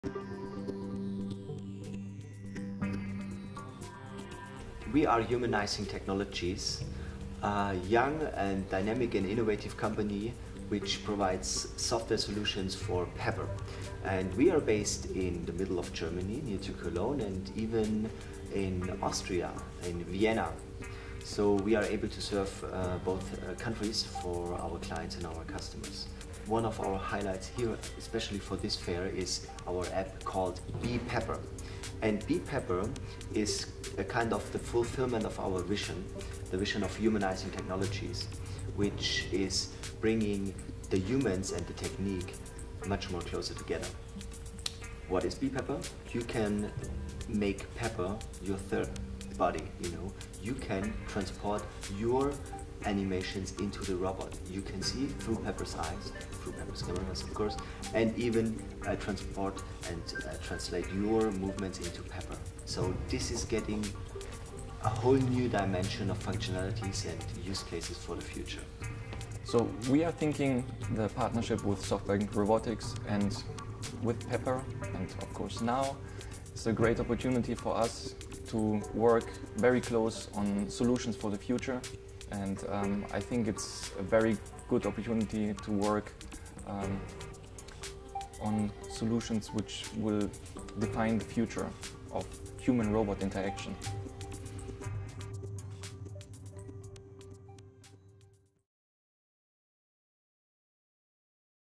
Humanizing Technology - Pepper World Paris 2017
During Pepper World Paris 2017, some of our Certified Partners were introducing a broad range of B2B applications and solutions developed for Pepper. These applications are answering several business needs in Retail, Banking, Hospitality or Healthcare. Discover them on video now.